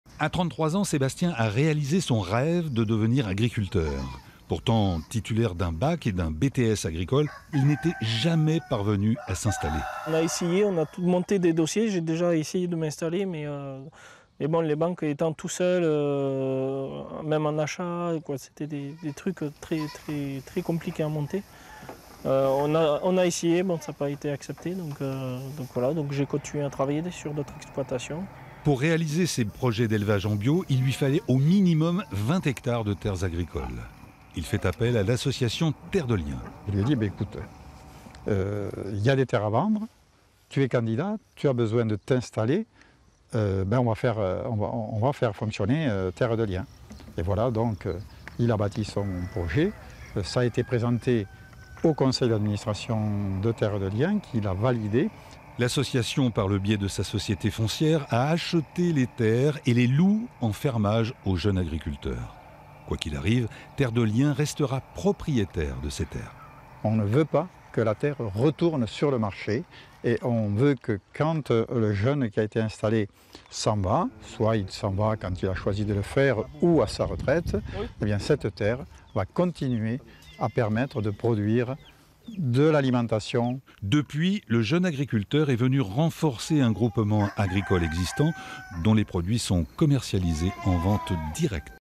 Aquitaine : agriculteurs, vous n'arrivez pas à vous installer ? "Terre de Liens" peut vous aider !
A chaque fois la même réponse des banques : non. Sébastien, lot-et-garonnais, n'y croyait plus. Le mouvement citoyen "Terre de Liens" lui a alors proposé d'acheter des terres pour lui, afin qu'il se mette en fermage et lance son activité dans le bio. Pari réussi.